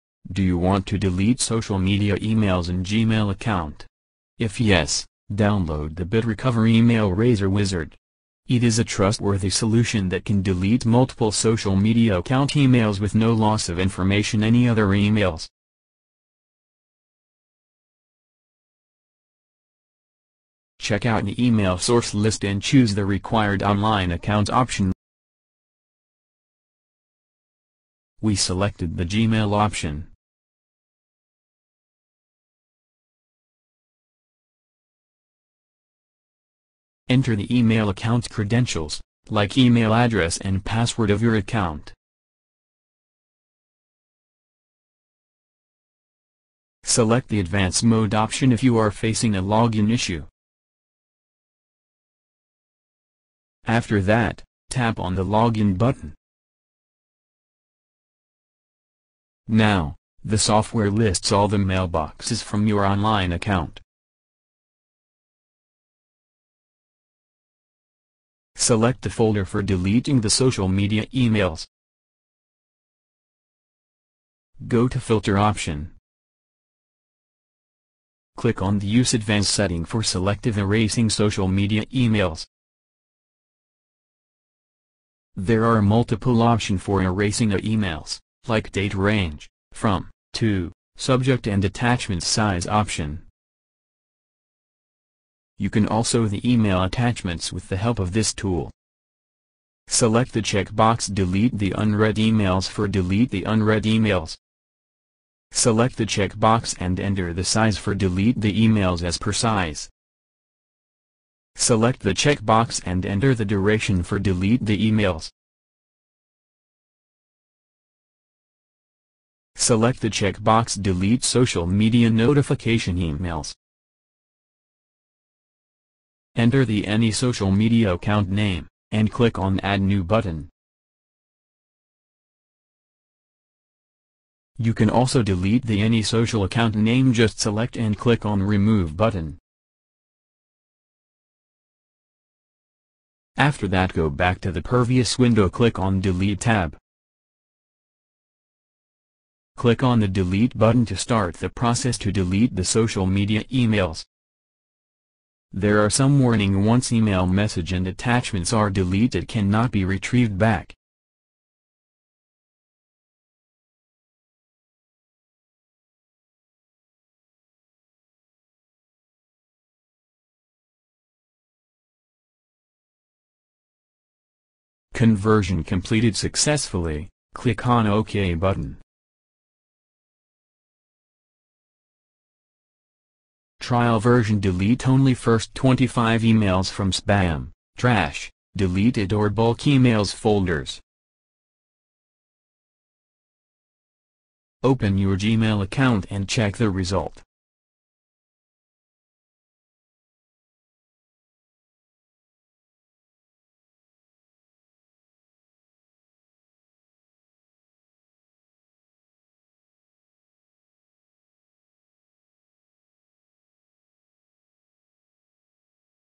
How to Delete All Social Emails in Gmail at Once – Simplest Solution
In this video we are going to discuss about how to delete social emails in Gmail in brief. Multiple users were seeking solution for this issue, but they are unable to find the correct solution.

On daily basis we receive a lot of emails on Gmail account. Due to which at one point we start facing storage related issues. In this case we usually search for those mails which are nit important for us. So, that we can delete them. Social emails are one such emails which are of less use. But user will face issue in clean gmail social emails if he follows manual solution.
So, for avoiding issues it is better to go for third party application.

Bitrecover EmailRaser Application is one such program which can be used by both technical and non – technical users. It will permanently delete all social emails in gmail at once. User will save their crucial time by using this application.

How to Use EmailRaser Application – Steps

Follow some simple steps for performing this task in few simple steps:

1. First, user have to install and run EmailRaser Application on their Windows OS.
2. After that, select the desired Gmail account and enter login credentials in the software interface.
3. Then, enable desired mailboxes of chosen email account from which you want to delete all email messages.
4. From the "Filter Options" section, enable the 'Use Advance Settings for Selective Erasing'.
5. Finally, click on the delete button. A pop – up box will appear containing warning message. Click on OK.
6. Now, software will start deleting the selected emails.

Timeline
0:00 – Introduction
0:17 – Select Gmail Account
0:35 – Enter Login Credentials
0:54 – Click on Login button
1:19 – Filter Options
2:27 – Click on Delete Button
2:42 – Deleting Process begins